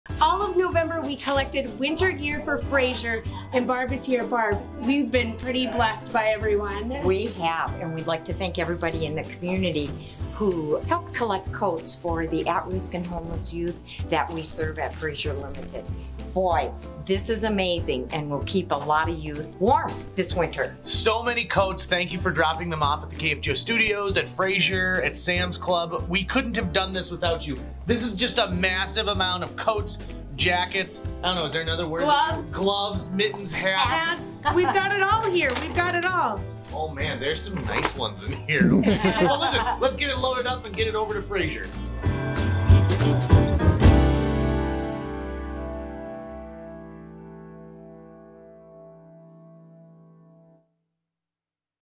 KFGO Coat Drop Off For Fraser
KFGO has been collecting coats from the community for Fraser LTD. Fraser Supports Children, Youth, and Adults on Their Life's Journey Towards Independence. Around $2,000 worth of winter apparel was donated! THANK YOU!